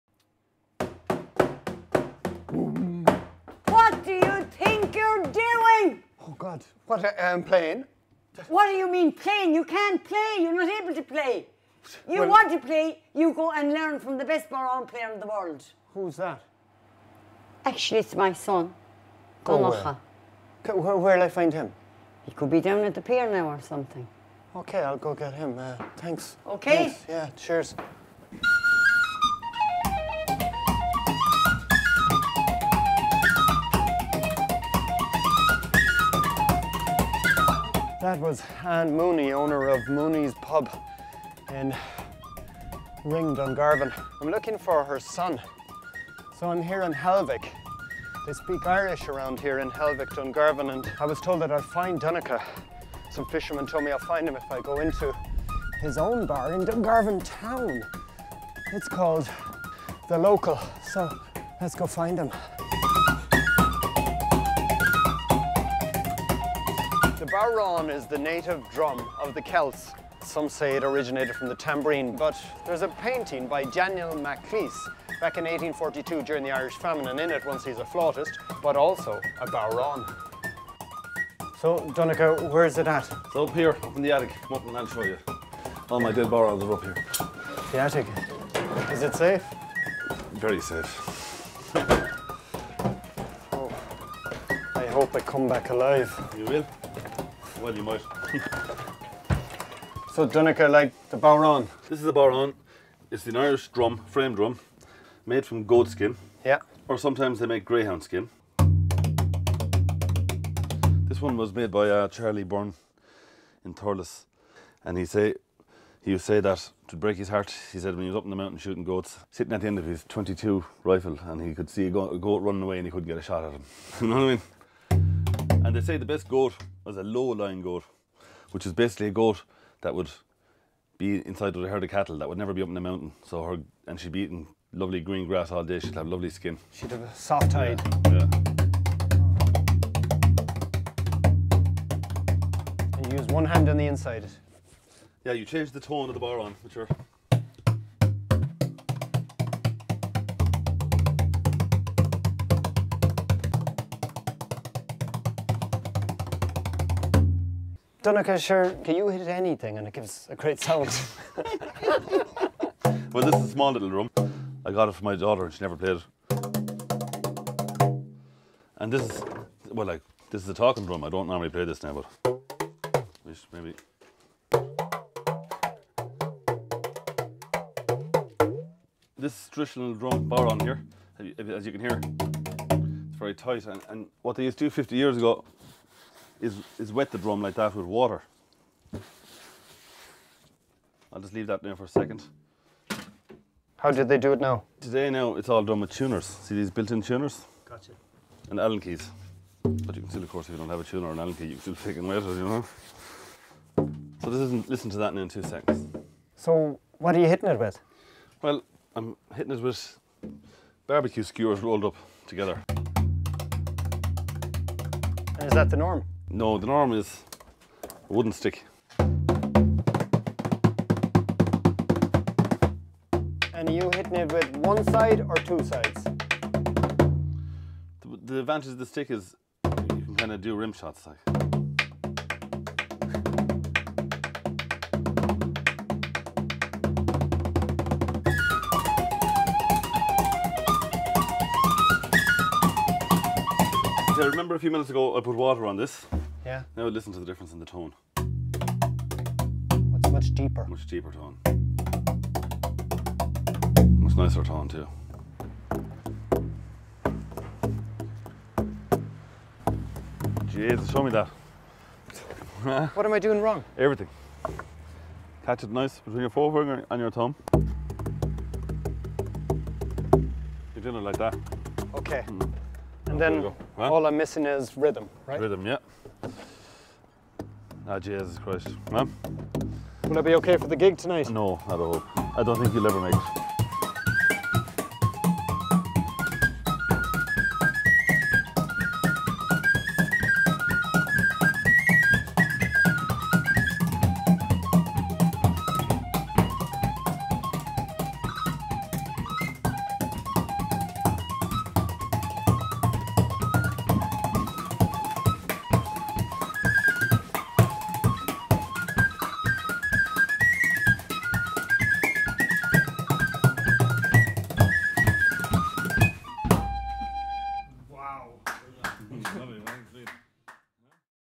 What Irish Do: The Bodhrán
In this episode, and Colin Carroll meet Donnchadh Gough. Donnchadh is a bodhrán maestro hailing from An Rinn in County Waterford, one of the last bastions of the native ‪‎Irish‬ language. Our intrepid reporter Colin Carroll gets schooled on the art of making and playing the bodhrán.